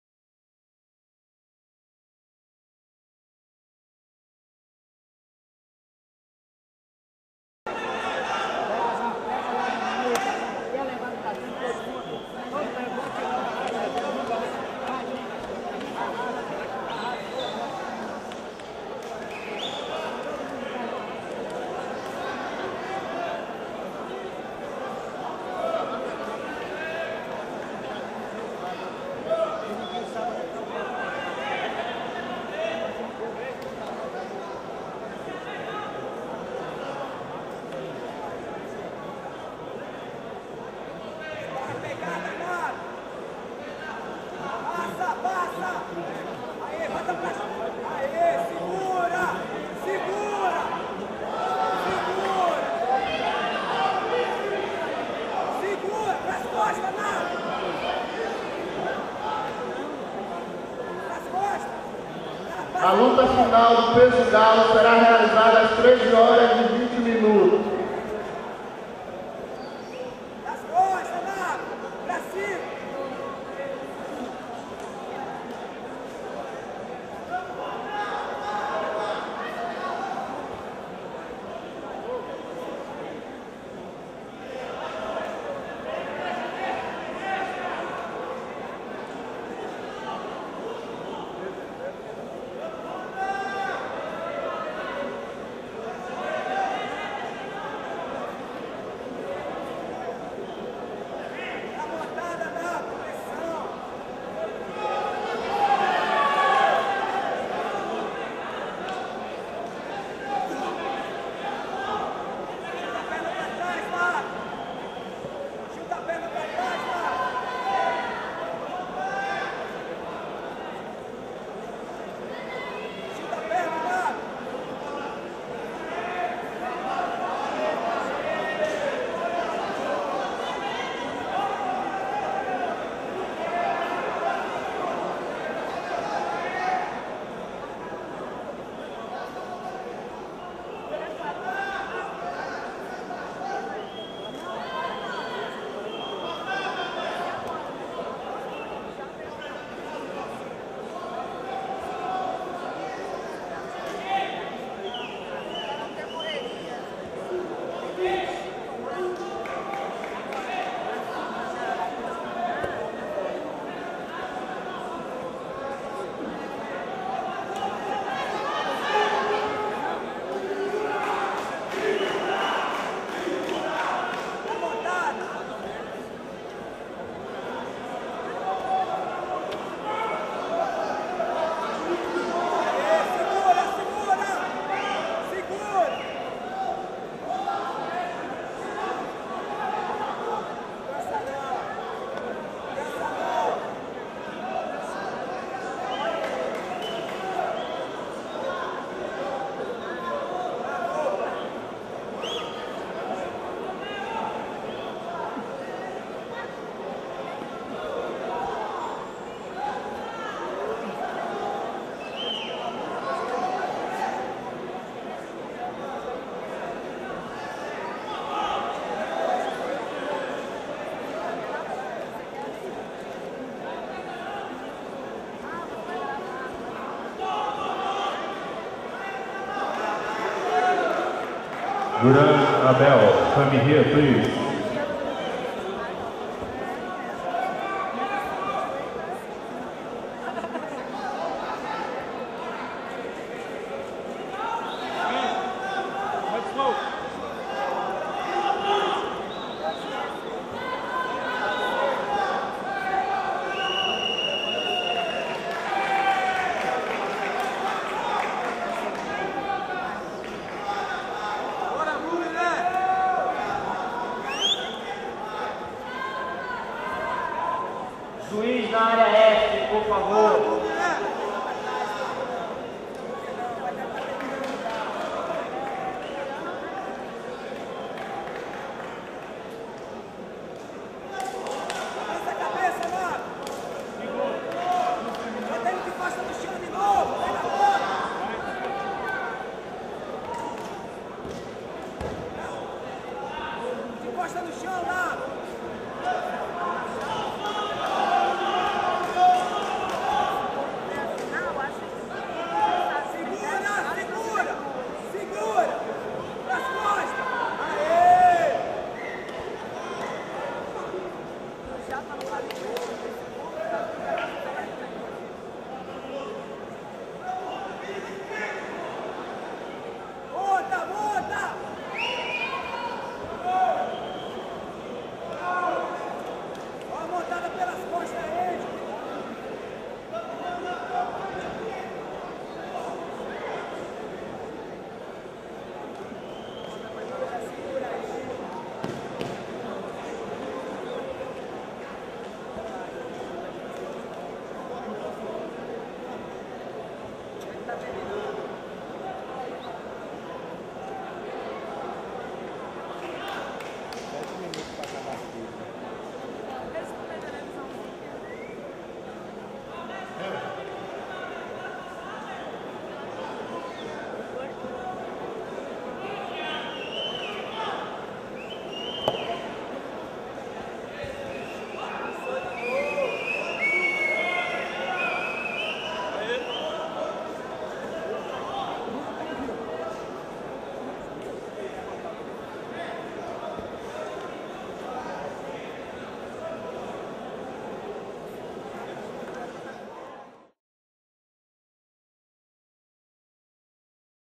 Maximiliano de Godoy x Carlos Eduardo by X-COMBAT
JIU-JITSU WORLD CUP 2004 - SALVADOR - CBJJO